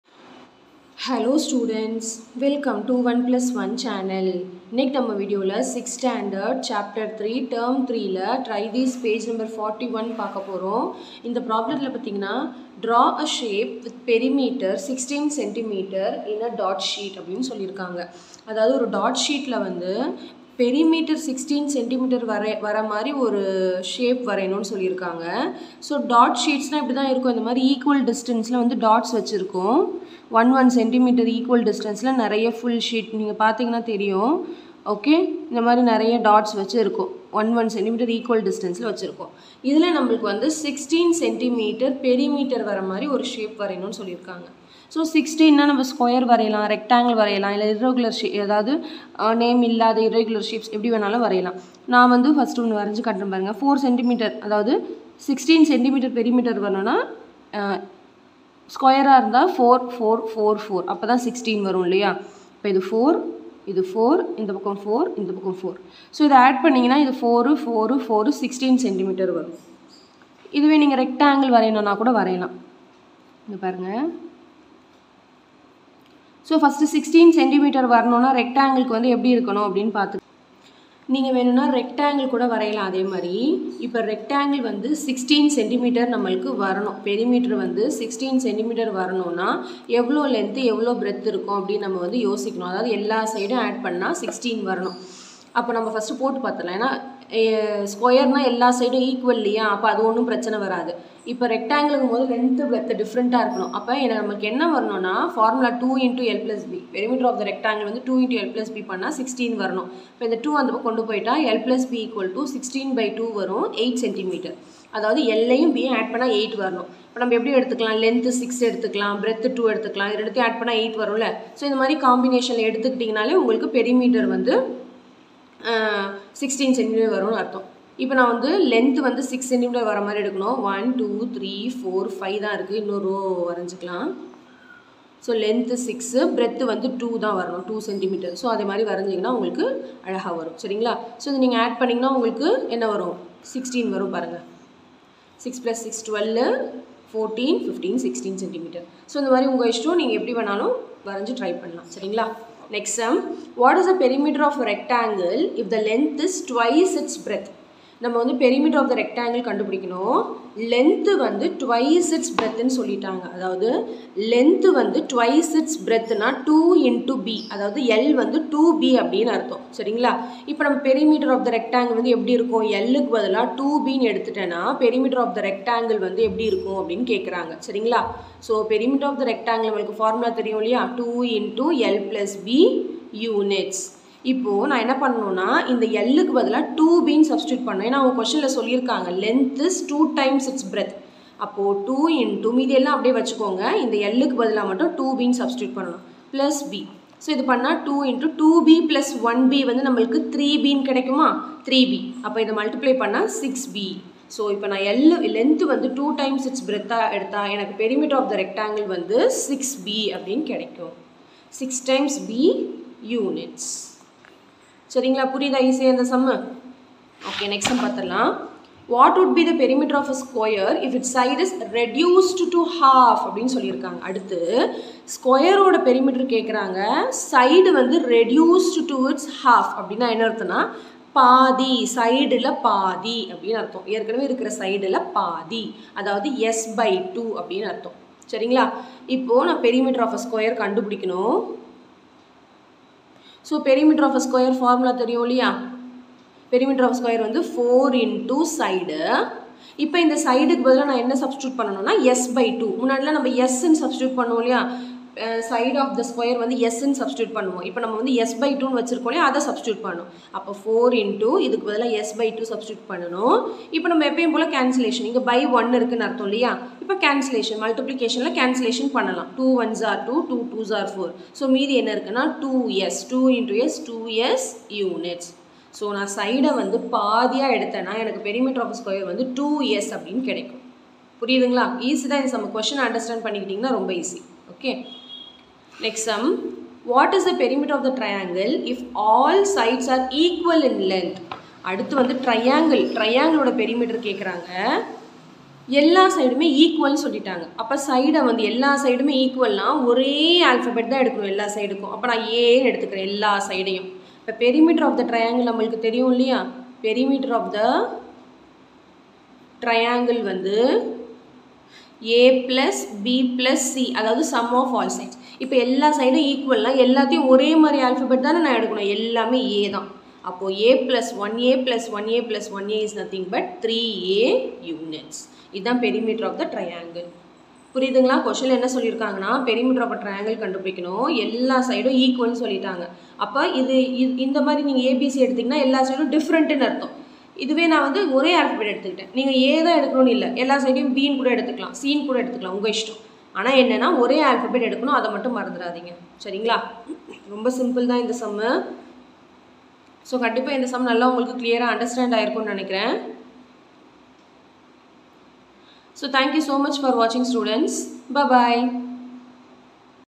6th Maths T-3 Ch-3 Perimeter and Area | Try these Page Number 41 | Samacheer One plus One channel
This video contains for 6th standard maths Term-3 Chapter-3 Perimeter and Area, Try these Page Number 41 complete solution.

My Another Channel 👇
Sangeetha Media